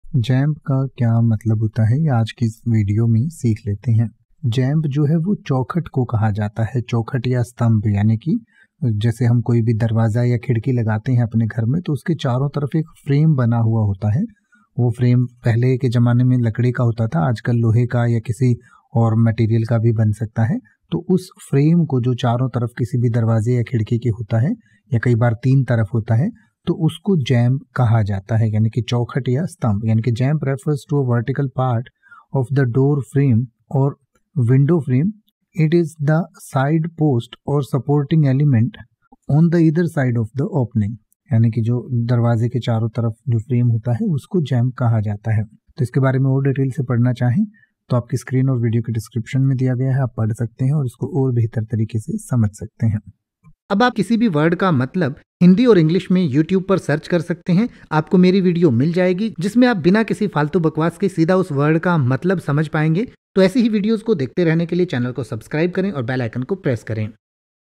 Jamb meaning in Hindi | Jamb ka matlab kya hota hai | English to hindi
इस वीडियो में आप Jamb का हिंदी में मतलब समझेंगे और साथ में Jamb का pronunciation भी सीखेंगें यानि की आप Jamb के अर्थ के साथ साथ ये भी सीखेंगें की Jamb को कैसे बोले या Jamb को बोलने का सही तरीका क्या है |

तो बस एक मिनट में सीखिए Jamb को|
Let’s learn Hindi Meaning of Jamb in detail.

Jamb का हिंदी में अर्थ होता है
चौखट, स्तंभ

Jamb refers to a vertical part of a door frame or window frame. It is the side post or supporting element on either side of the opening.

The carpenter measured the width of the jamb to ensure the new door would fit properly.
यह सुनिश्चित करने के लिए कि नया दरवाज़ा ठीक से फिट होगा, बढ़ई ने चौखट की चौड़ाई मापी।

The window's jambs were painted to match the trim of the room.
खिड़की के चौखटों को कमरे की साज-सज्जा से मेल खाने के लिए चित्रित किया गया था।

Jamb को इन उदाहरणों से और अच्छे से समझ सकते हैं